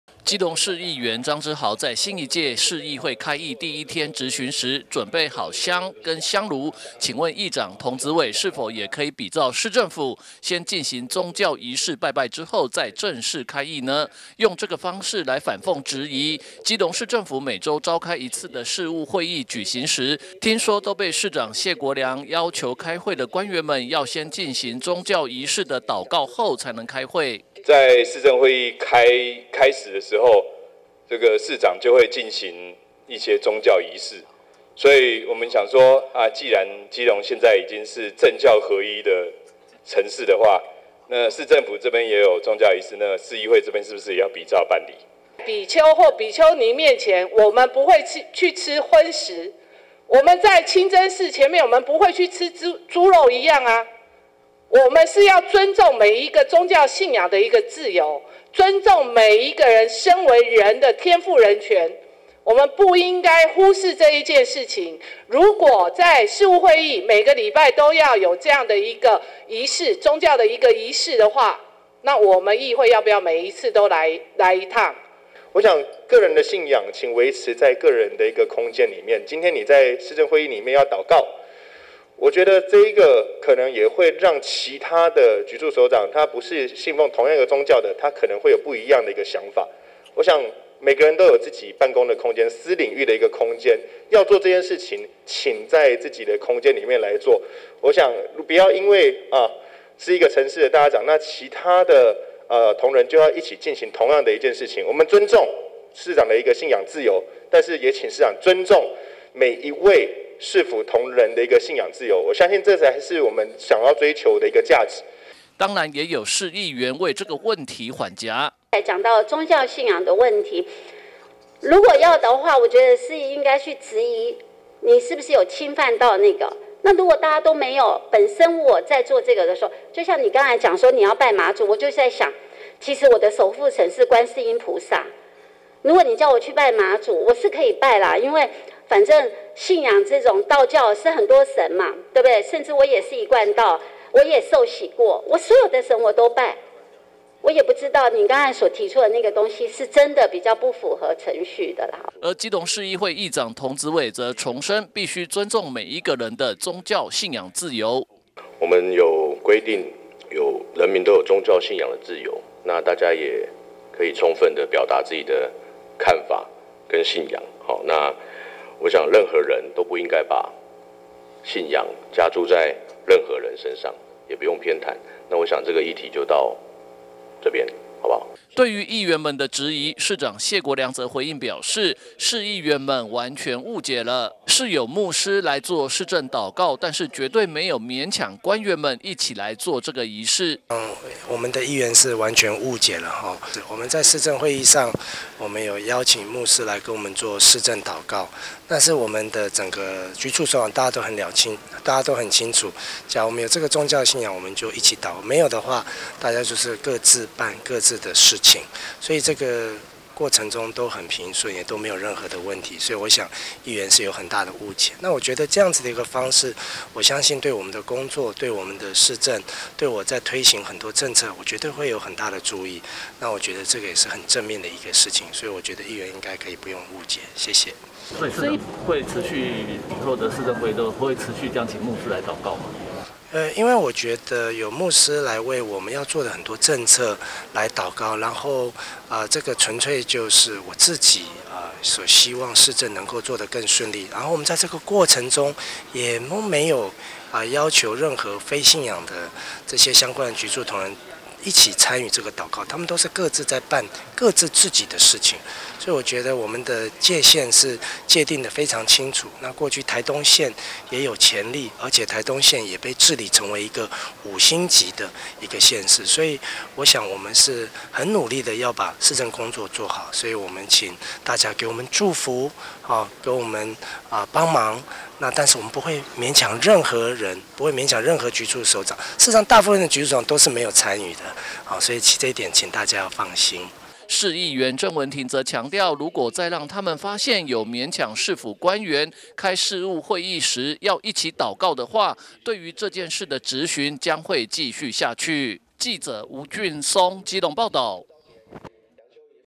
基隆|童子瑋|謝國樑|張之豪|鄭文婷|張顥瀚|呂美玲|市務會議|政教合一|禱告|市府市務會議要求禱告？議員批政教合一？
市府市務會議要求禱告？議員批:政教合一？
市長:議員完全誤解 有牧師禱告但絕無勉強

(中嘉吉隆新聞記者吳俊松/基隆報導)新一屆基隆市議會23號召開第一次臨時會時，就有市議員質疑，基隆市政府每週召開一次的市務會議舉行時，聽說都被市長謝國樑要求要先進行宗教儀式的禱告？對此，市議員批評市政府是否已經變成”政教合一”的體制？市長謝國樑則回應表示，市議員誤解了，是有牧師來禱告，但是沒有強迫官員們一起做這個儀式！

基隆市議員張之豪在新一屆市議會開議第一天質詢時，準備好箱跟香爐，請問議長童子瑋，是否也可以比照市政府先進行宗教儀式拜拜之後再正式開會呢？用這方式來反諷質疑，基隆市政府每周召開一次的市務會議舉行時，聽說都被市長謝國樑要求要先進行宗教儀式的禱告後才能開會？

當然也有市議員為這個問題緩頰！

基隆市議會議長童子瑋則重申必須尊重每一個人的宗教信仰自由！

對於議員們的質疑？市長謝國樑則回應表示，市議員完全誤解了，是有牧師來做市政禱告，但是絕對沒有勉強官員們一起做這個儀式！

市議員鄭文婷則強調，如果再讓她們發現有勉強市府官員開市務會議前要一起禱告的話，對於這件事的質詢將會繼續下去！